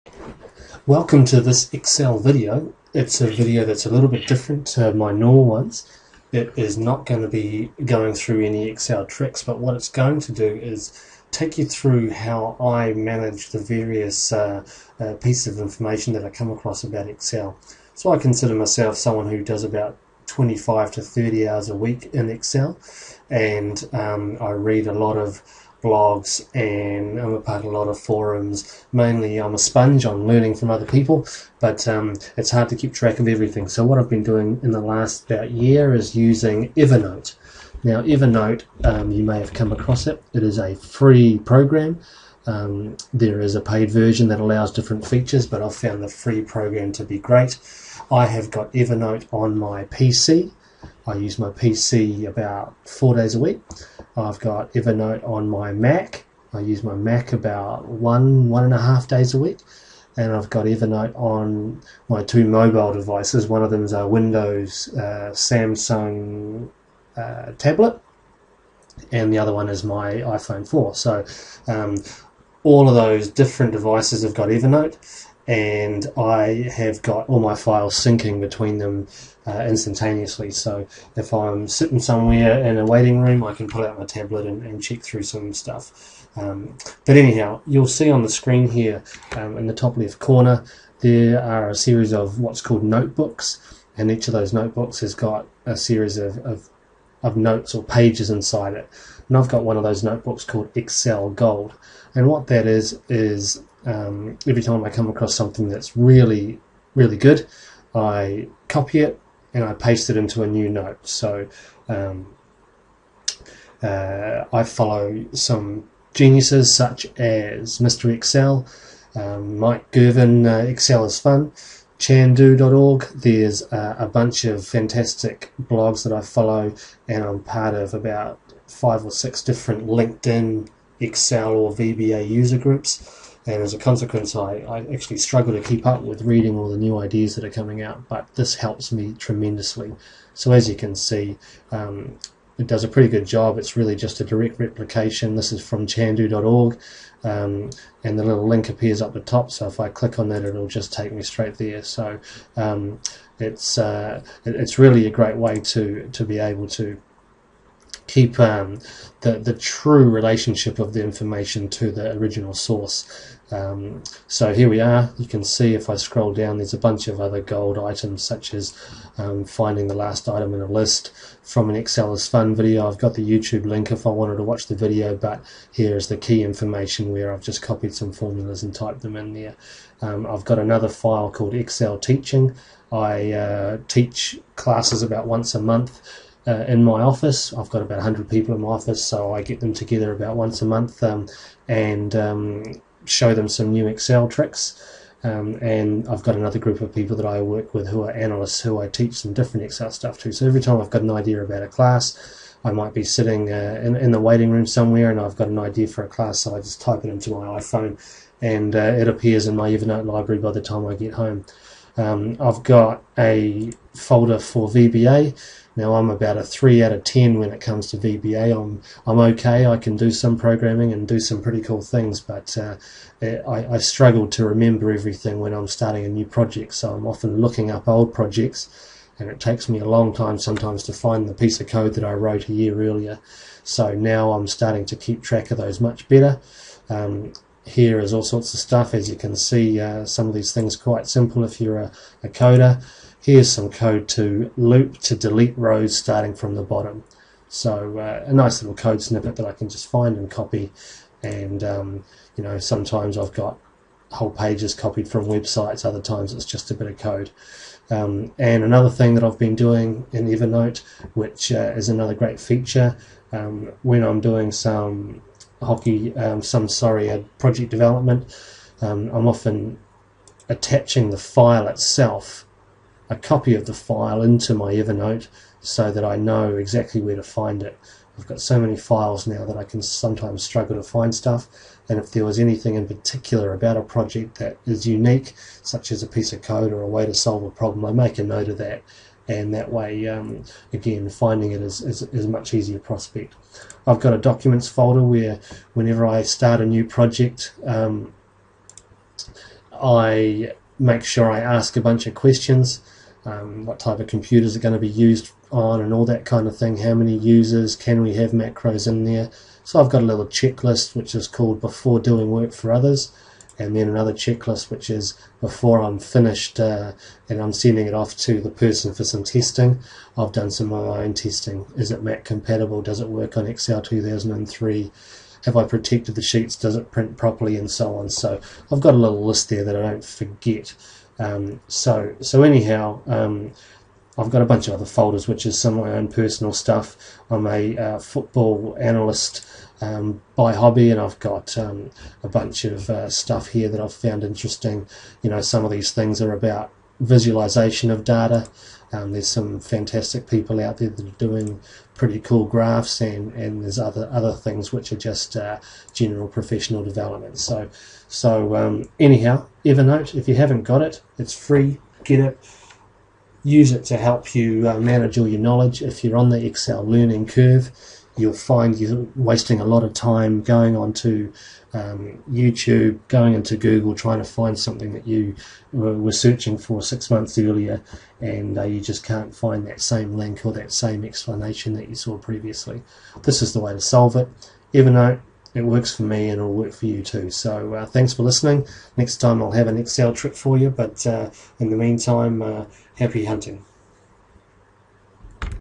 EAF#25 - Using Evernote to be Awesome at Excel
Description
Excel Tips for Fitness Trainers and Sport Scientists
Use functions and built in features of excel 2010 to make your life easier
This video explains how i use the free version of Evernote on four different devices (a PC, a Mac, a tablet and an iPhone) to keep track of all my excel projects, ideas and important tips and knowledge